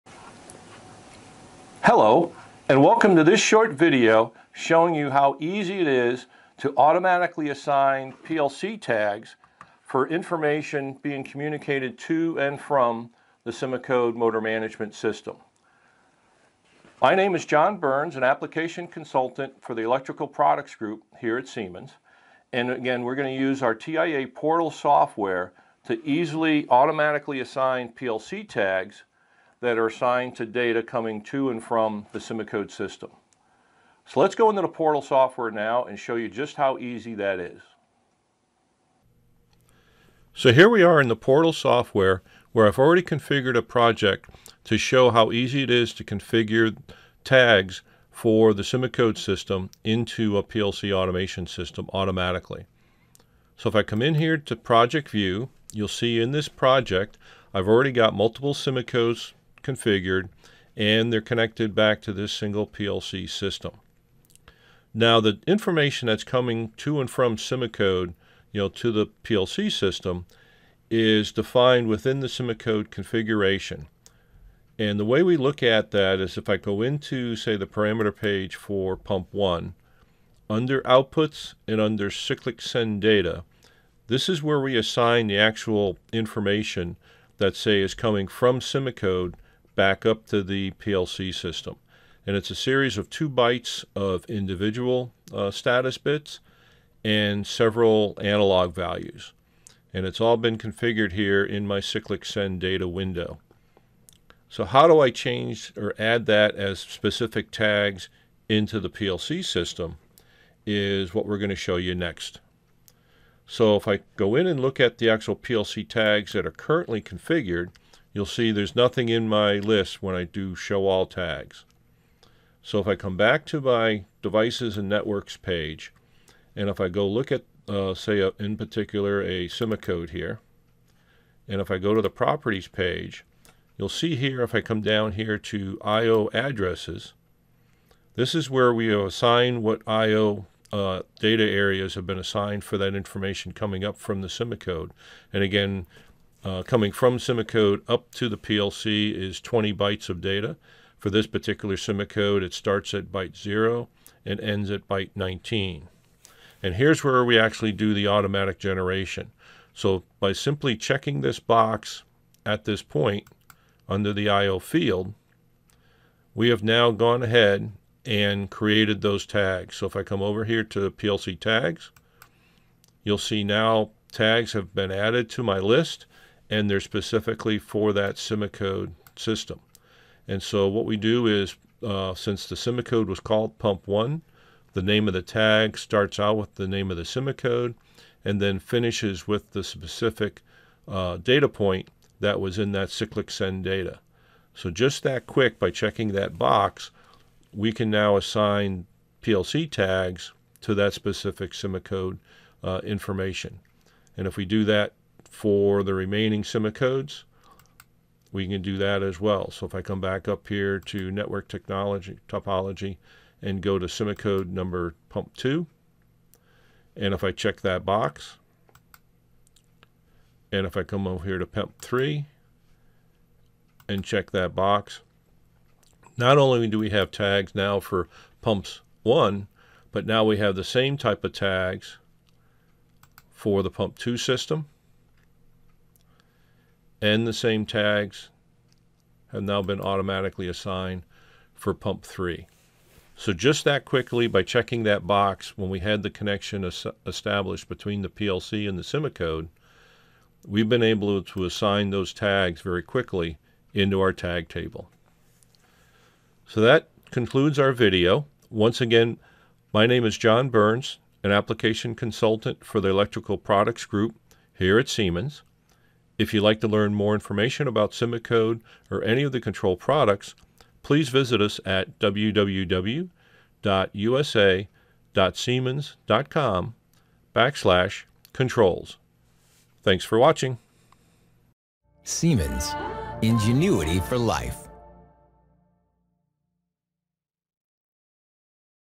How to automatically assign PLC tags for SIMOCODE pro in TIA Portal
This video goes through  how to automatically create PLC tags for a SIMOCODE pro Motor Management System using the TIA Portal software.